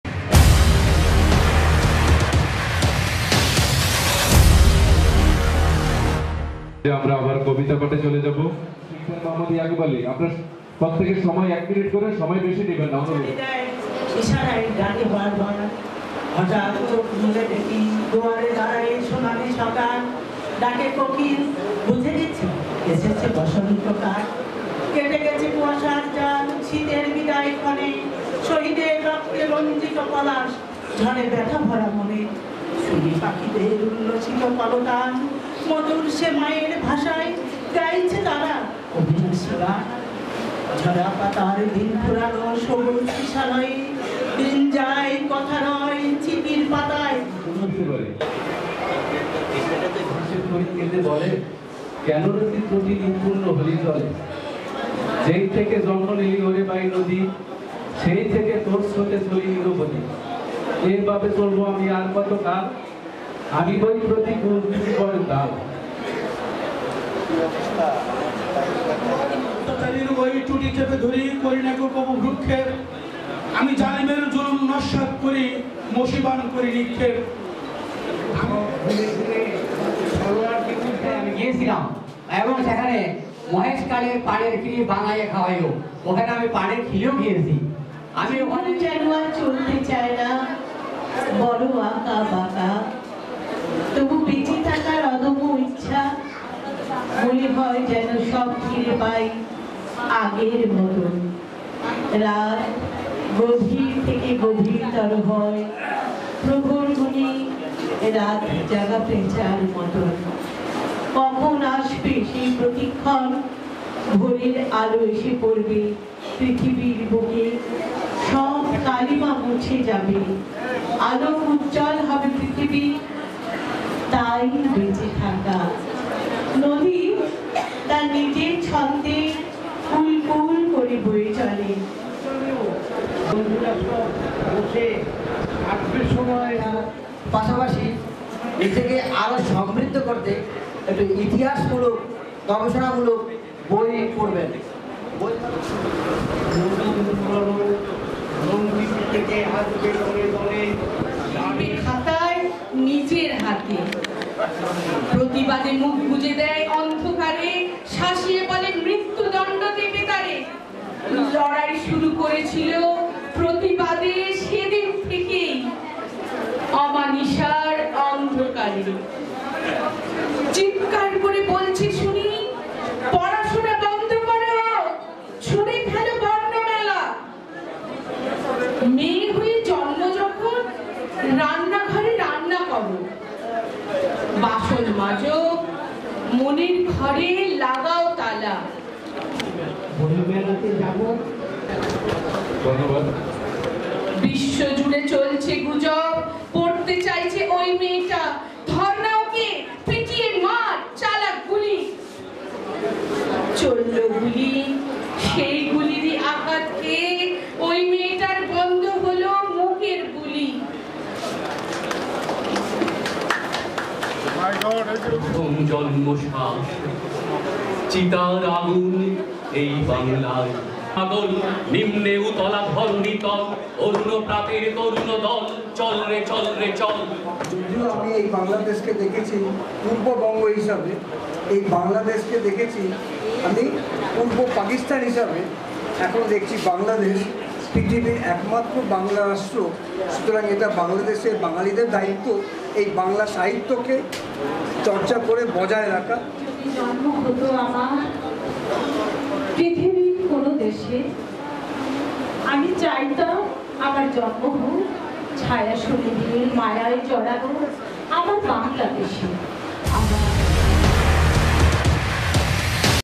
আন্তর্জাতিক শুদ্ধ সাহিত্যচর্চা নিয়ে দুই বাংলার মিলনমেলা ও কবিতা পাঠ /Reunion poetry of two Bengals.
আন্তর্জাতিক শুদ্ধ সাহিত্যচর্চা নিয়ে দুই বাংলার মিলনমেলা ও কবিতা পাঠ /Meeting and poetry reading of two Bengals on international pure literary practice.

               Khalidur Rahman
              Navidur Rahman